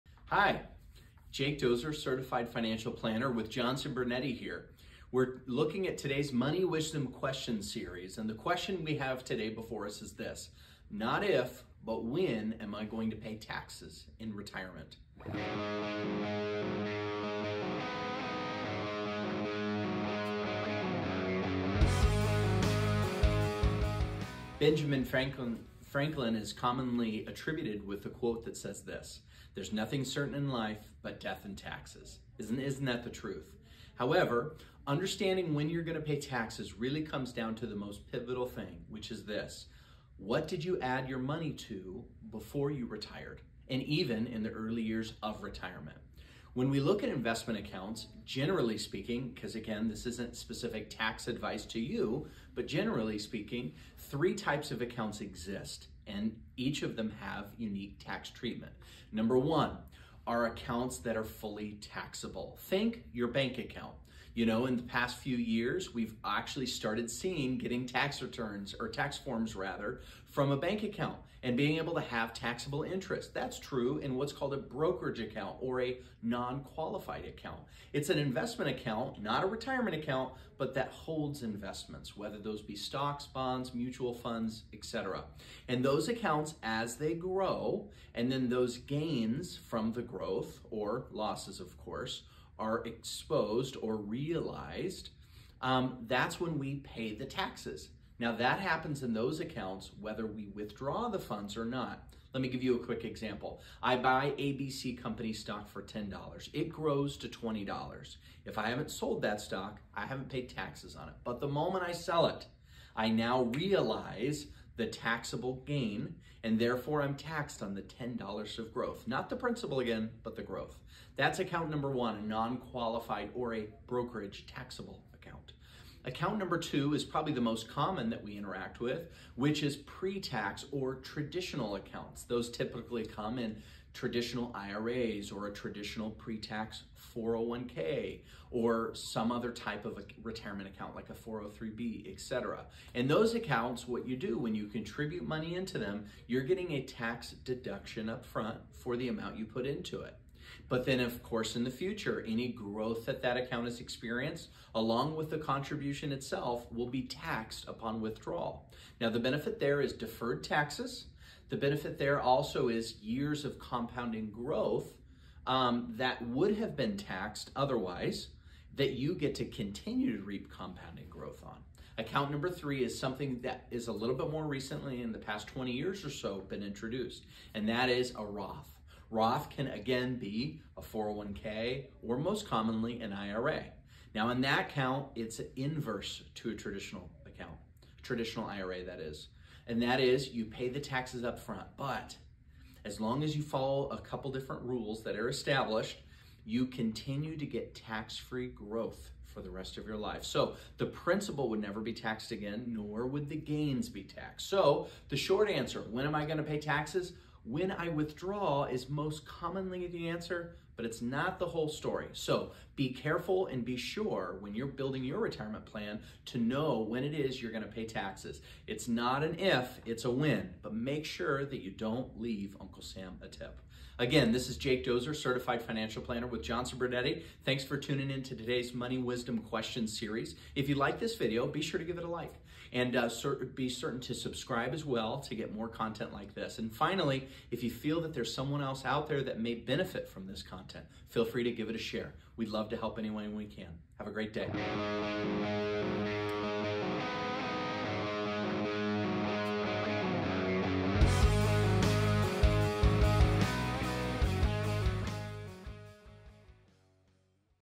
Not If, but When, Will I Pay Taxes in Retirement?
As you plan for your retirement, it's important to consider all aspects of your financial future. While many people focus on saving and investing for retirement, one area that often gets overlooked is taxes.

The reality is that taxes will likely still be a part of your life even after you retire. The question is not if you will pay taxes in retirement, but when.

Jacob Doser, CFP® is here to share more on this topic on the Money Wisdom Question Series.

Get More from us!

Need Financial Guidance? We help people just like you plan for and enjoy retirement.

Subtitles are auto-generated.